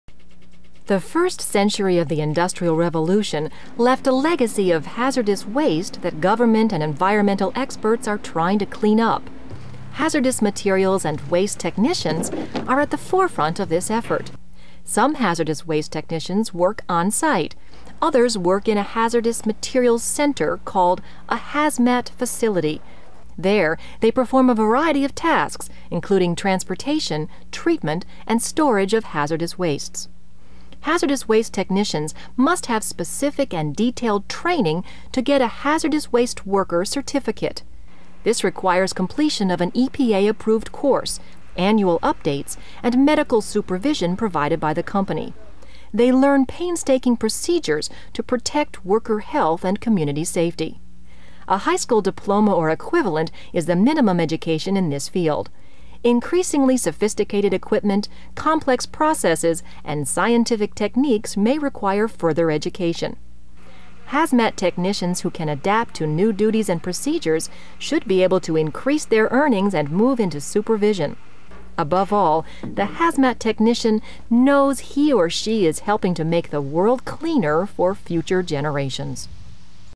Hazardous Materials Removal Workers
Identify, remove, pack, transport, or dispose of hazardous materials, including asbestos, lead-based paint, waste oil, fuel, transmission fluid, radioactive materials, contaminated soil, etc. Specialized training and certification in hazardous materials handling or a confined entry permit are generally required. May operate earth-moving equipment or trucks.